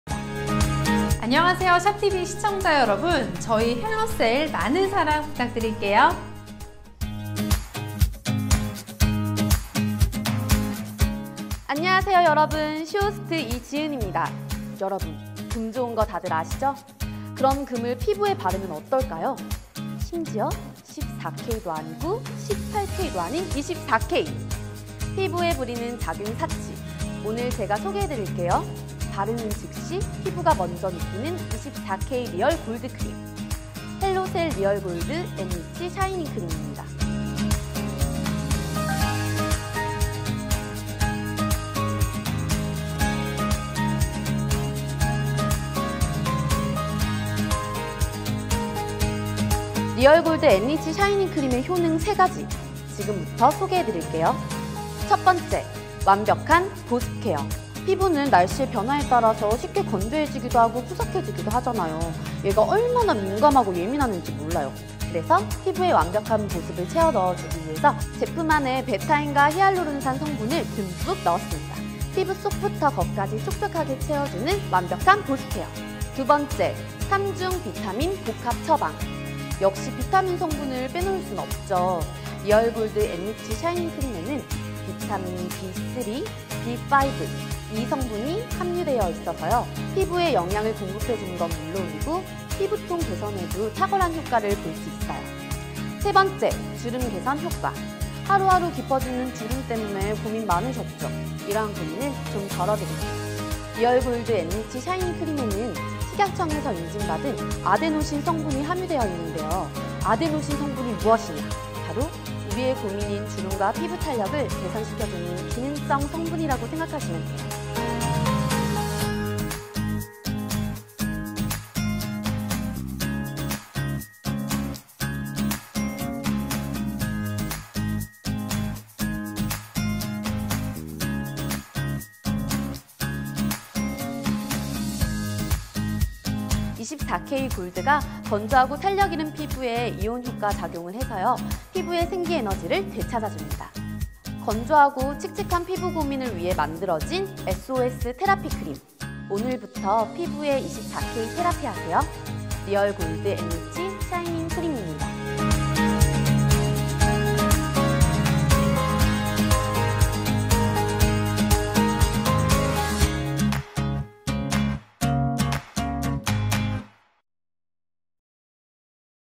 K-Beauty Derma Skincare Hellocell | Shop TV
Shop TV welcomes Hellocell with skincare products that helps skin defense and restore from air and environment pollution damage. or send a message to order.